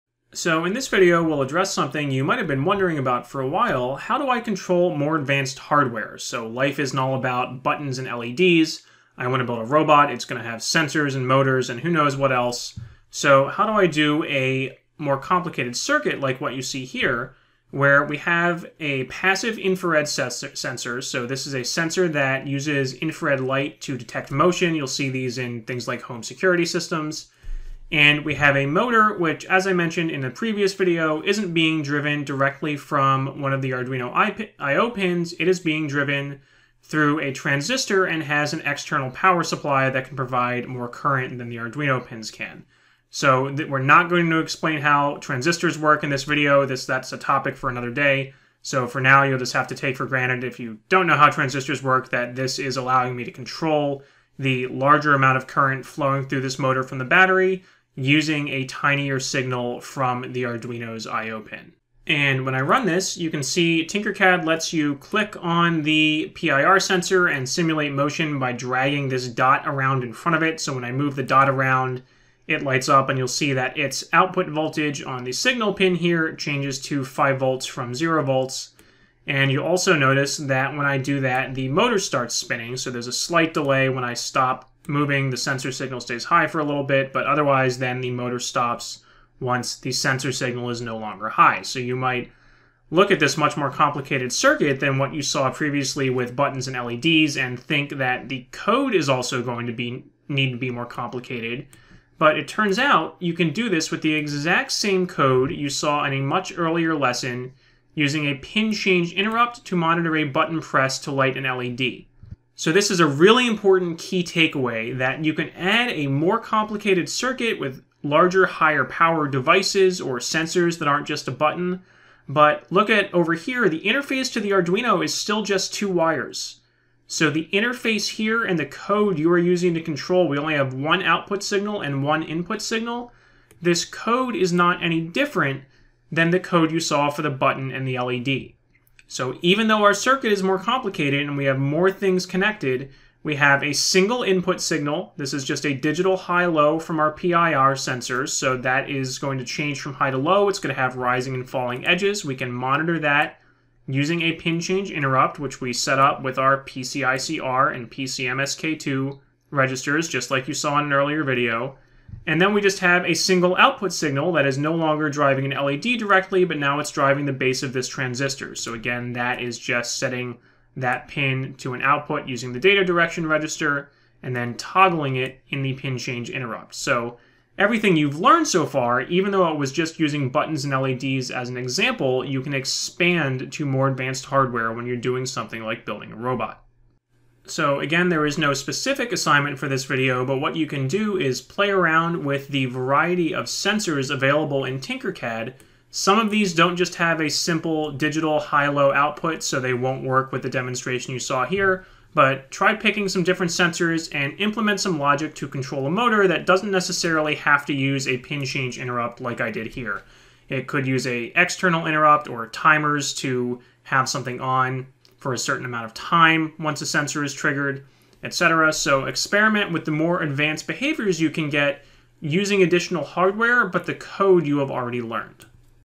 Tinkercad + Arduino Lesson 15: Motors and Sensors
This video shows how to use a PIR sensor to control a motor, using the same code you used earlier to control an LED using a button and pin change interrupts. It is part of a larger series of videos showing how to program an Arduino (Atmega329p) in Tinkercad Circuits. This content is taught in MAE 3780 - Mechatronics at Cornell University's Sibley School of Mechanical and Aerospace Engineering.